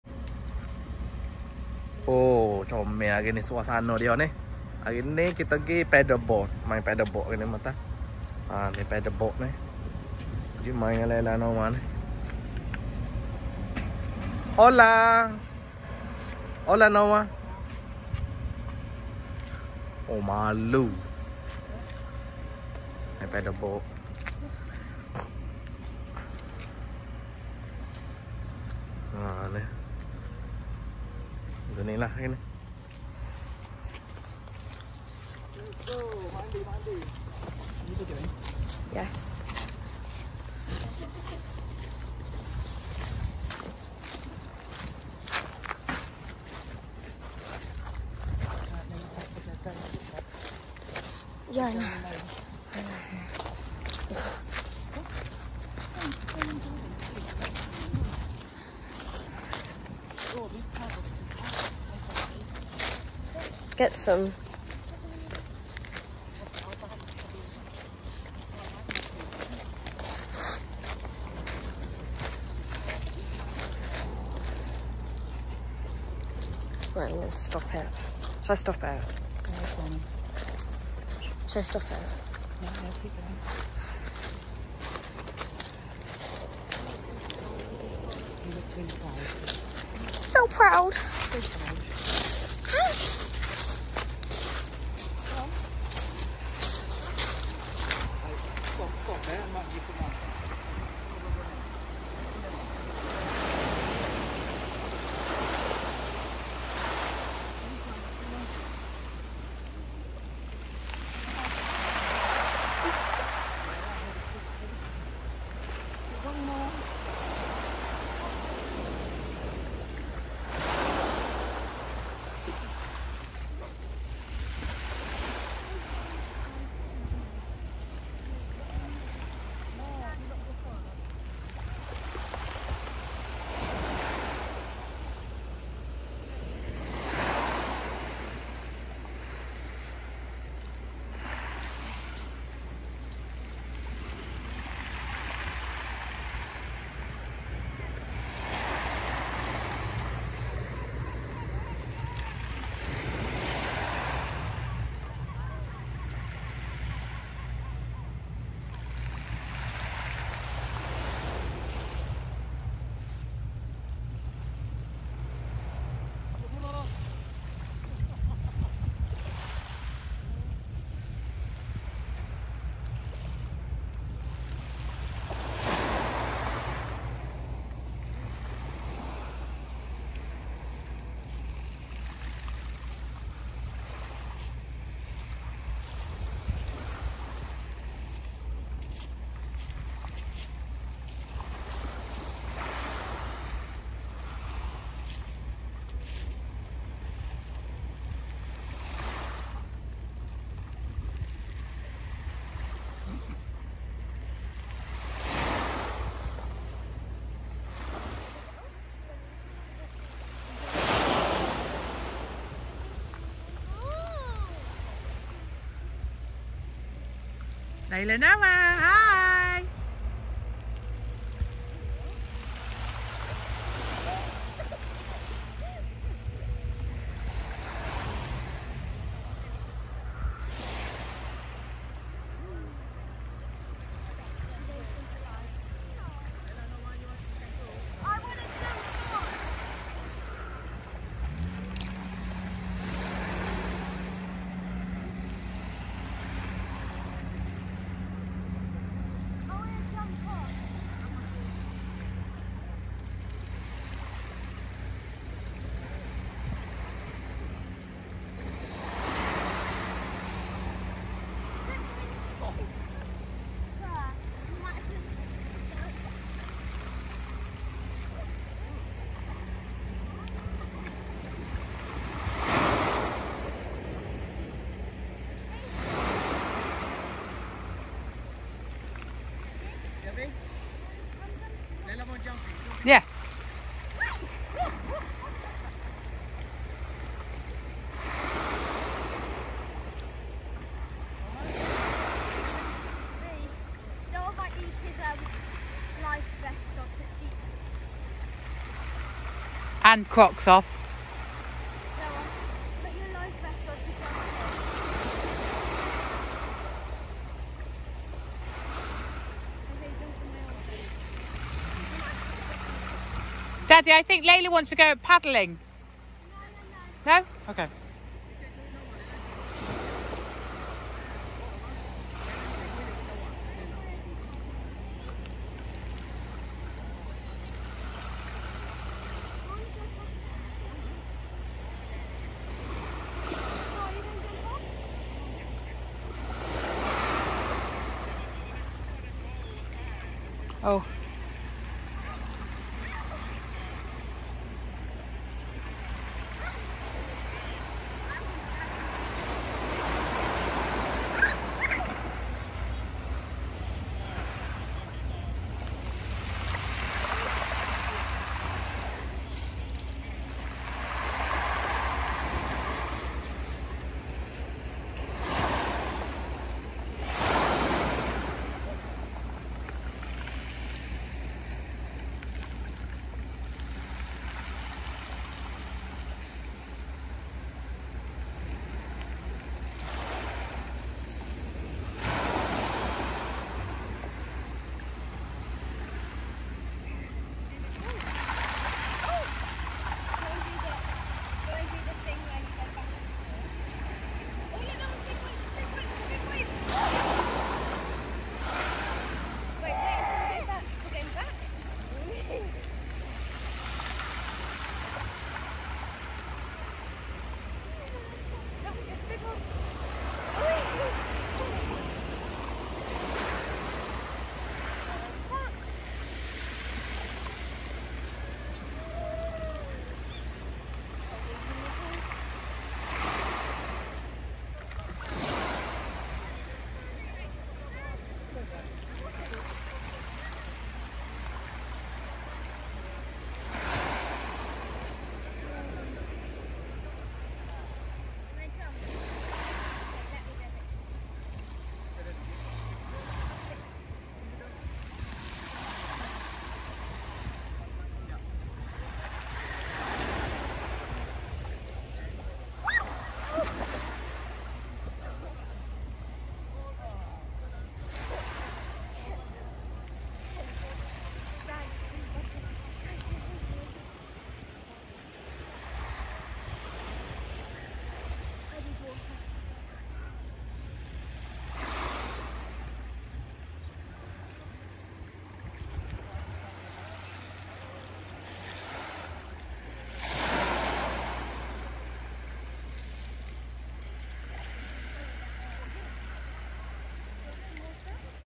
Paddle boarding @playa de Santiago
Bercuti di La Gomera ,Canaries Island Spain 🇪🇸